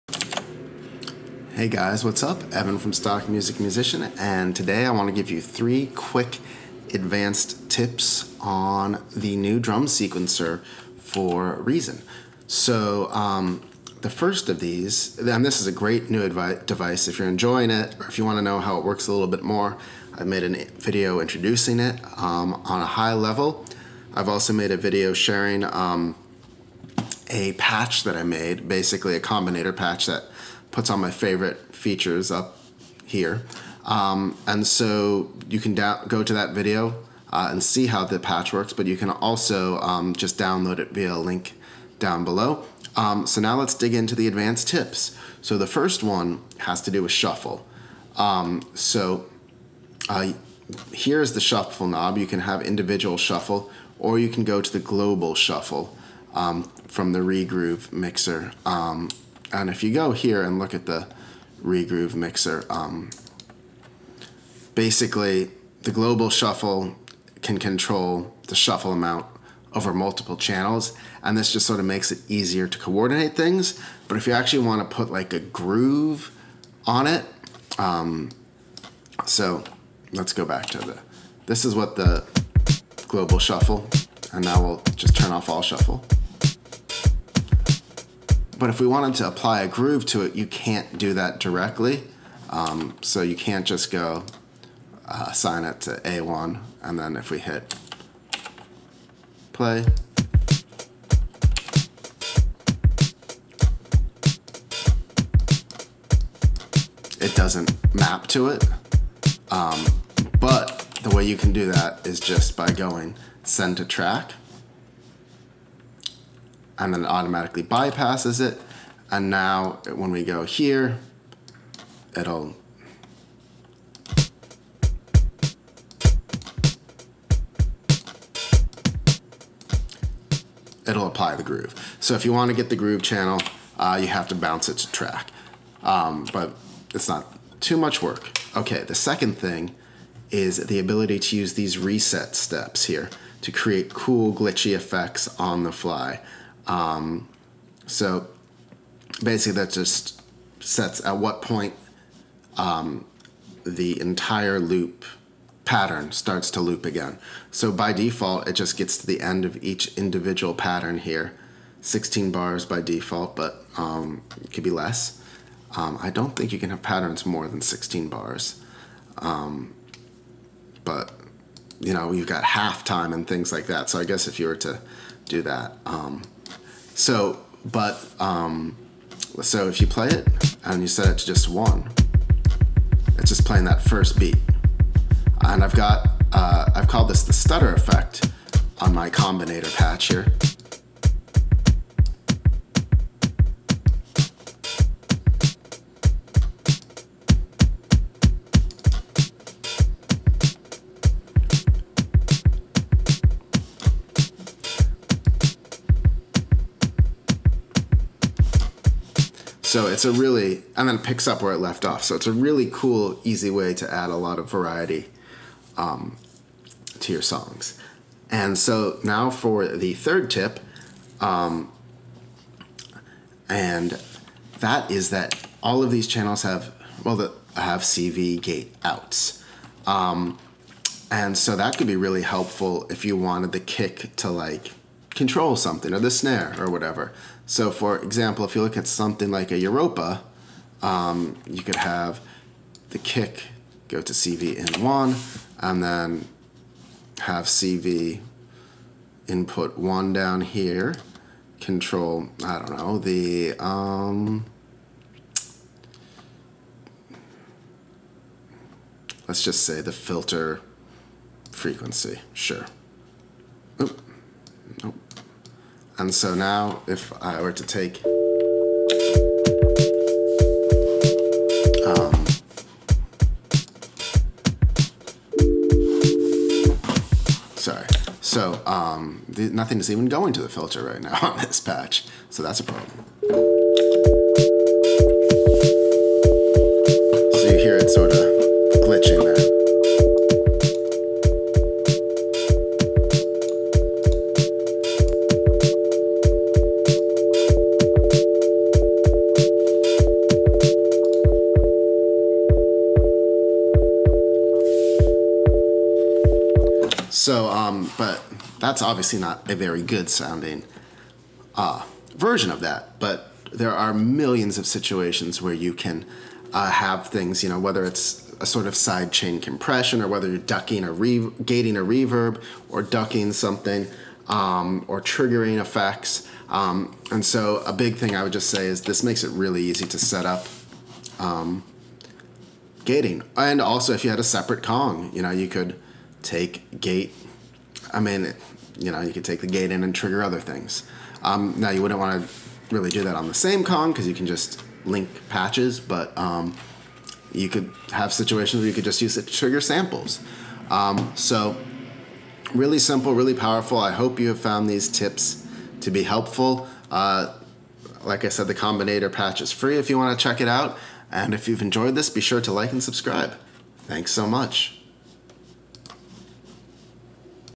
3 Advanced Tips for Propellerheads Reason Drum Sequencer (Reason 10 Beat Making)
Want to get the most from the Reason Drum Sequencer? Here's 3 advanced tips for Propellerhead Reason's new drum sequencer to have you making beats like a pro.

I'll show you how to get advanced grooves, how to easily create stutters, and how to use the CV out for easy creative sound design.